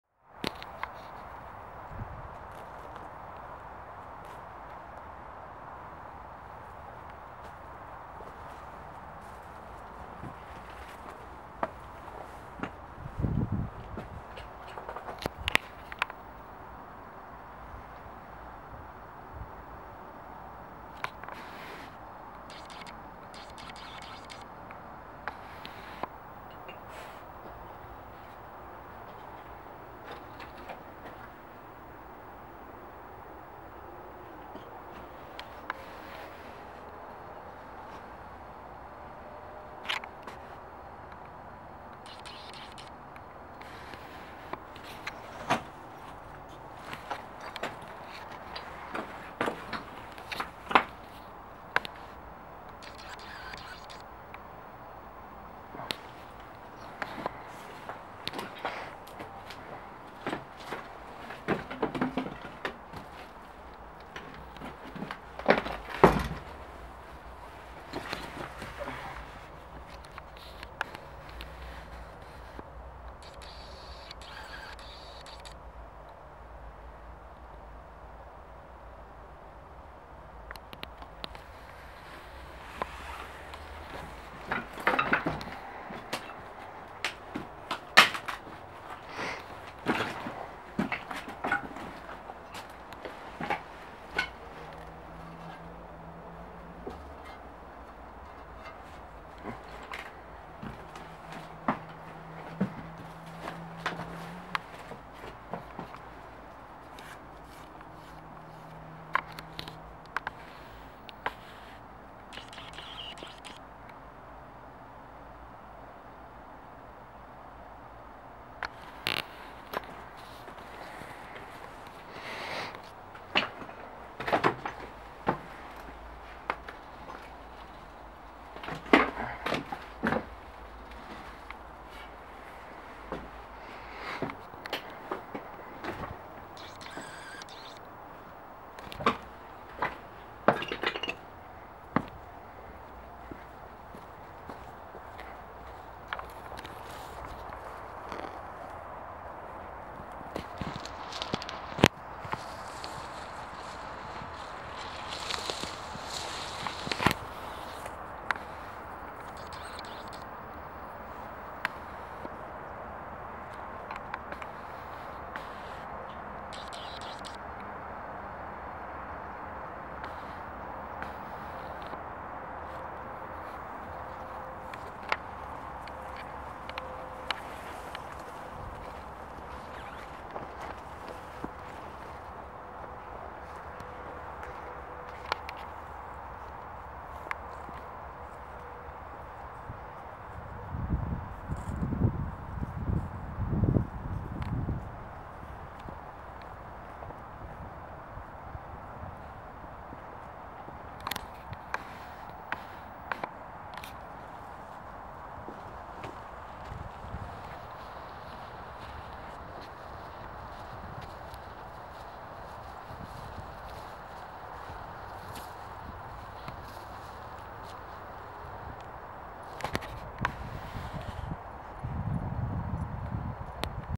verlassenes Pumpenhäuschen teil 2 bereits abgebrannt
hier mal der 2 teil des verlassenen pump Häuschen (Deutschland urbex Expedition das ich am 14 Juni 2013 hochgeladen habe hier der link dazu